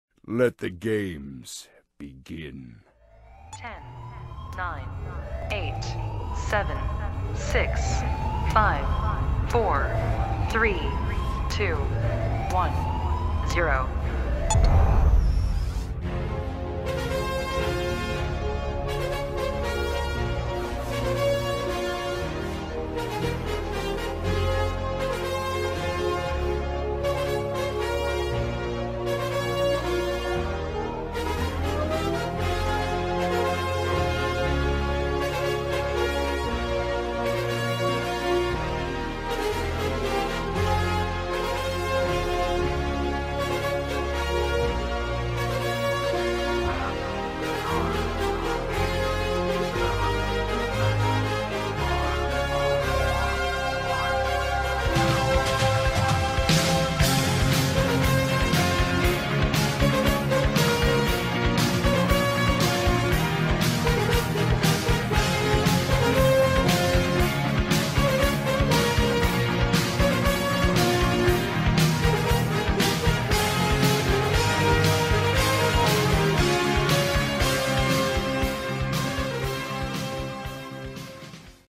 Promofilmpje: Landstede Volleybal - MOK Rovinj (06-11-2014)
Aanstaande donderdag 6 november (2014) speelt Landstede Volleybal tegen MOK Rovinj. Deze wedstrijd staat in het teken van het CEV-cuptoernooi! Support Landstede Volleybal aanstaande donderdag om 19.30 uur in het Landstede Sportcentrum!